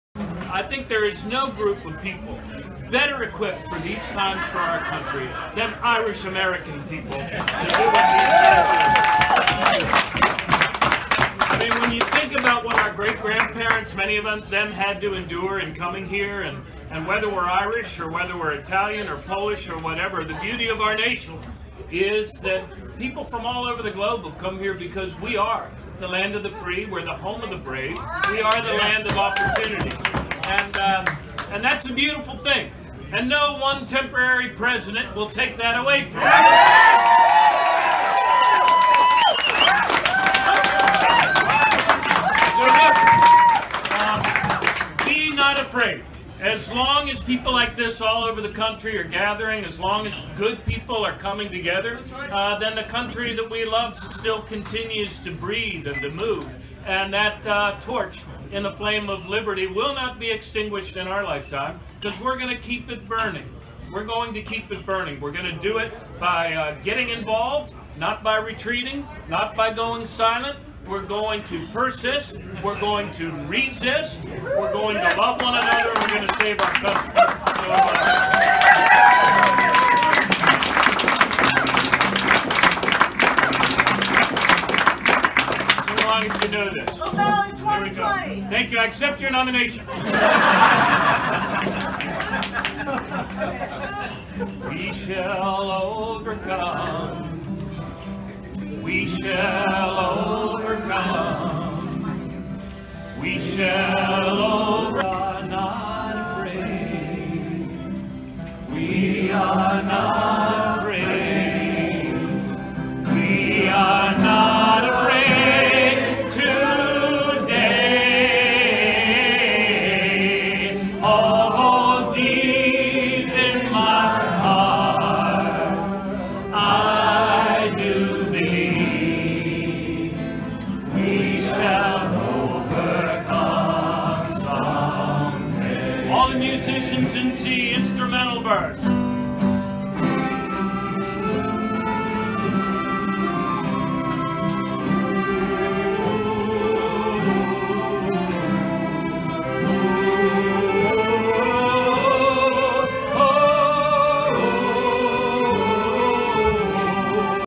Martin O'Malley at Sanctuary Session in Baltimore
3/5/2017: There is no group of people better equipped for these times for our country than Irish American people. When you think about what our great-grandparents, many of them had to endure when coming here and whether we’re Irish or whether we’re Italian or Polish or whatever, the beauty of our nation is that people from all over the globe have come here because we are the land of the free, the home of the brave, we are the land of opportunity. And that’s a beautiful thing. And no one temporary President will take that away from us.